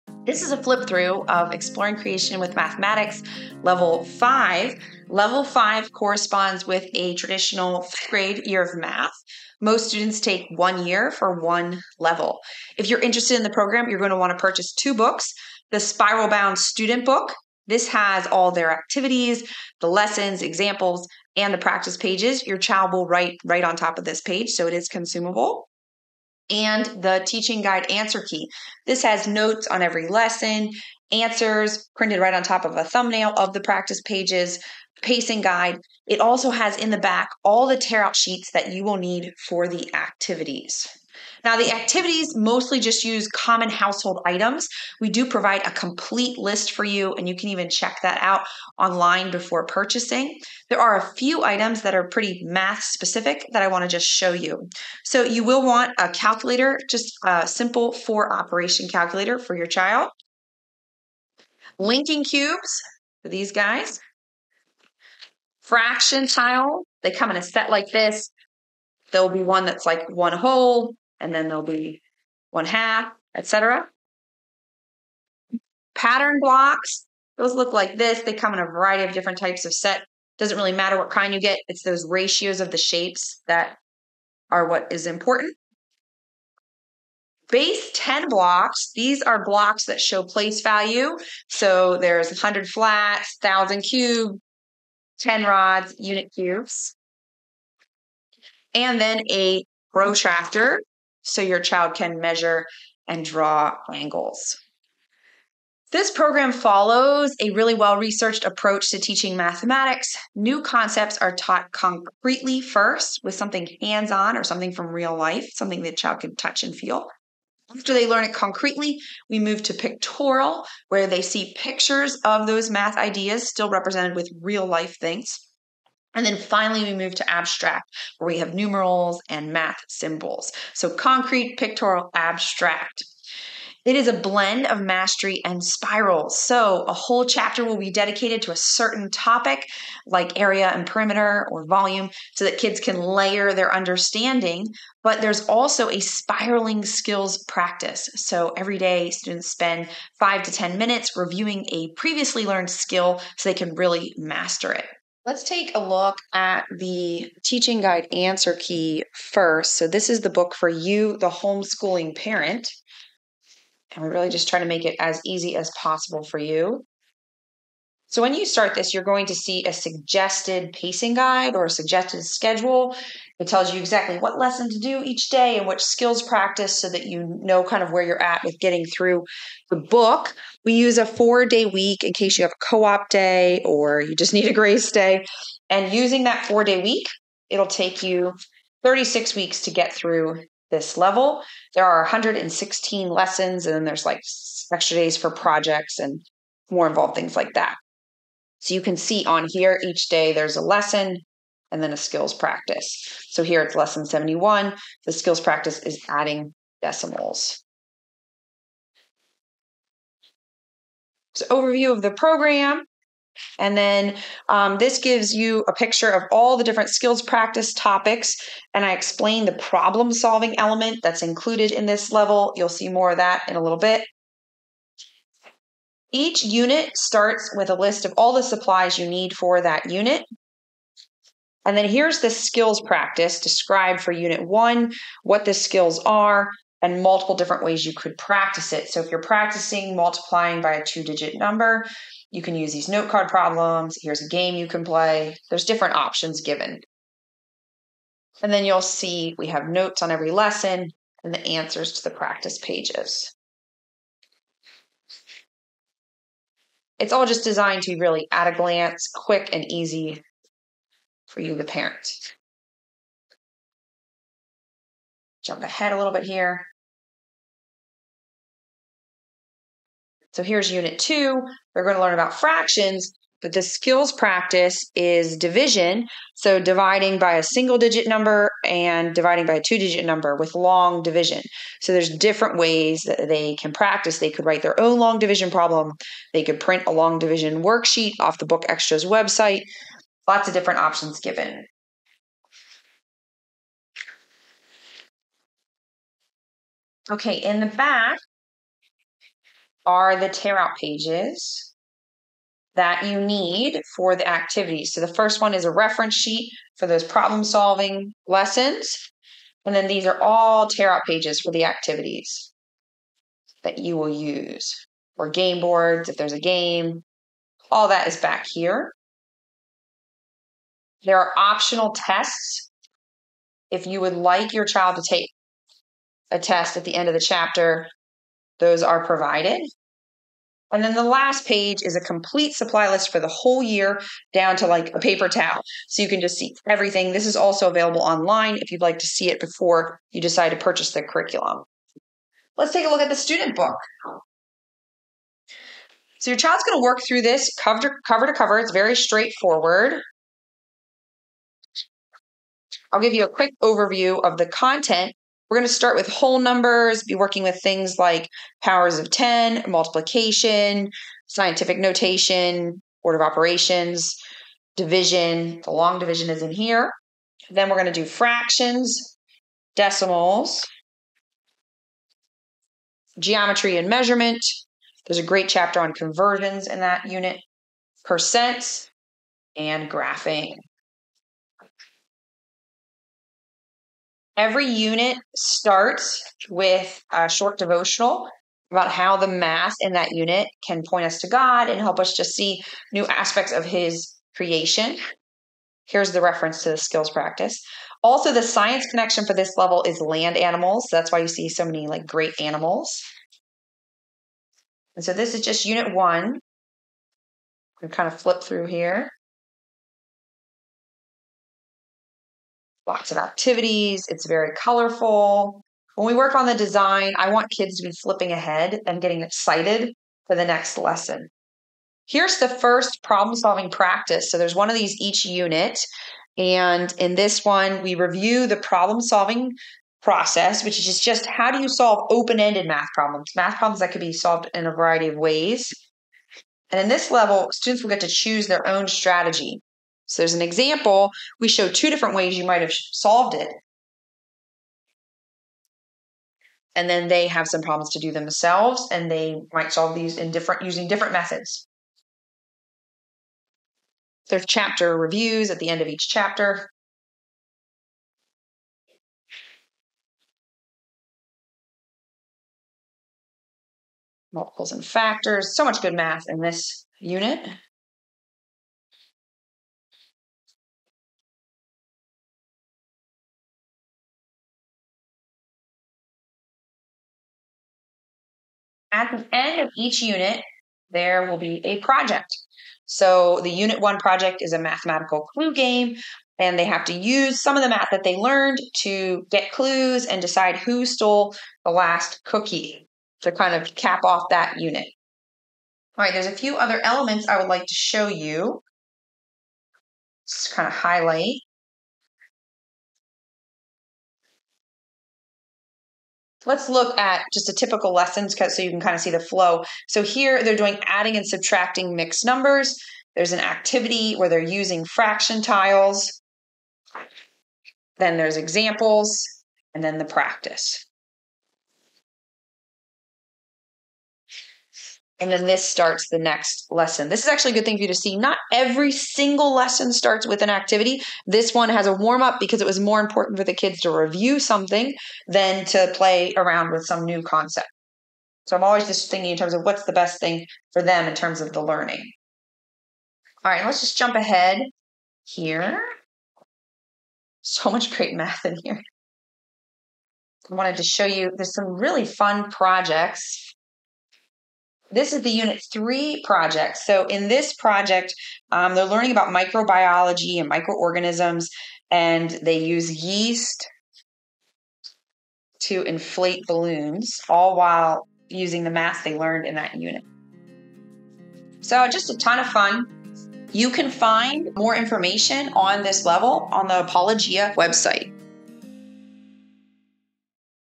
Exploring Creation with Mathematics, Level 5 Flip-through
Join Kathryn Gomes, the author of Exploring Creation with Mathematics, for an inside look at Level 5.

In this textbook, students will learn about whole numbers, fractions, decimals, geometry & measurement, percentages, and graphing. This hands-on, interactive math curriculum is filled with activities, practice problems, projects, and games that will help students comprehend lessons. Students will also see the connections between math, science, business, art, our world, and our universe. Most importantly, students will begin to see how the study of math points to our Creator.

Chapters
00:00:00 Introduction
00:00:41 Supply List
00:01:45 Approach
00:02:35 Look Inside, Teaching Guide & Answer Key
00:05:42 Look Inside, Student Workbook